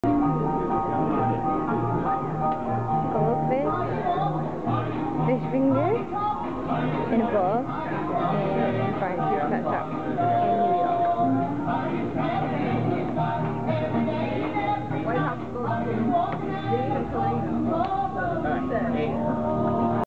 Banksy fishy fingers
fish fingers form Banksy's New York pet shop exhibition in Greenwich Village 2008